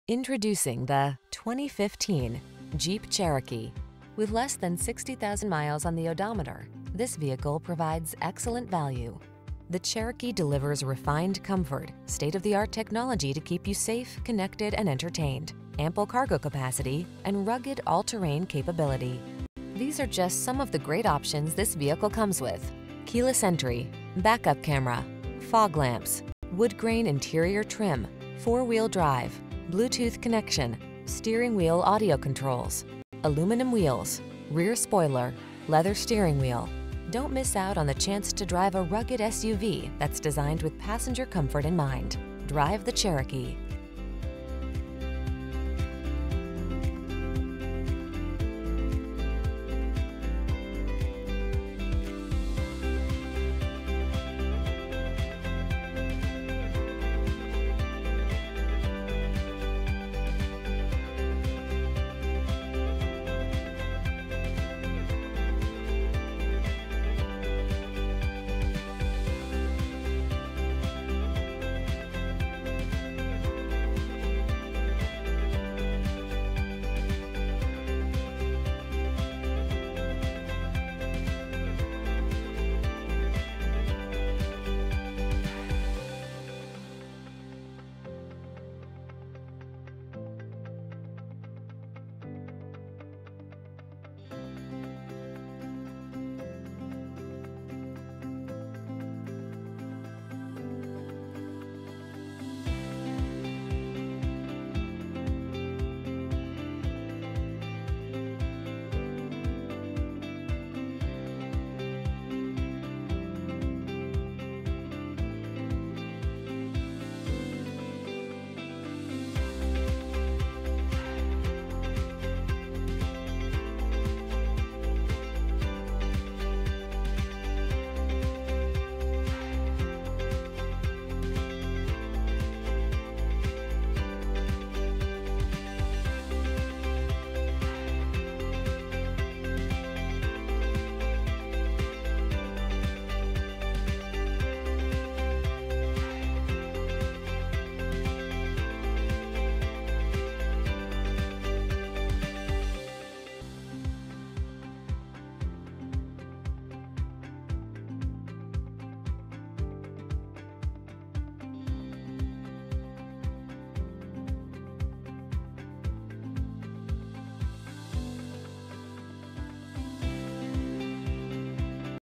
2015 Jeep Cherokee Vestal, Binghamton, Ithaca, Johnson City, Big Flats, NY HYU57997
Deep Cherry Red Crystal Pearlcoat Used 2015 Jeep Cherokee available in Vestal, New York at Miller Auto Team. Servicing the Binghamton, Ithaca, Johnson City, Big Flats, NY area.

Deep Cherry Red Crystal Pearlcoat 2015 Jeep Cherokee Latitude 4WD 9-Speed 948TE Automatic 2.4L I4 PZEV MultiAirOdometer is 34221 miles below market average!